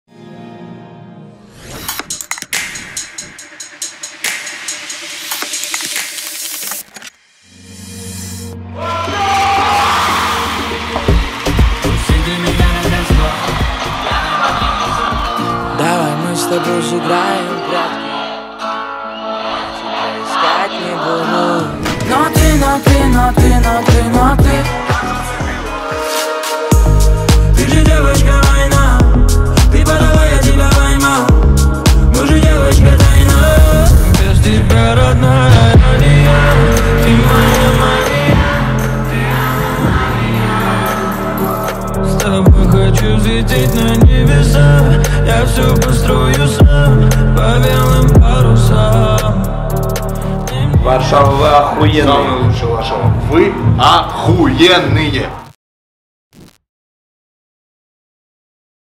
Концерт HamMali & Navai в Варшаве (05.10.2019) [ prod. by Shot Space Production ]
Отчет с концерта HamMali & Navai в городе Варшава. Сделано специально для Secrets Of Music | Event Agency
Над видео работали:
Camera: @rostishka.one , kot_volshebnik
Editing/VFX: @rostishka.one
cooperation: @shot.space.production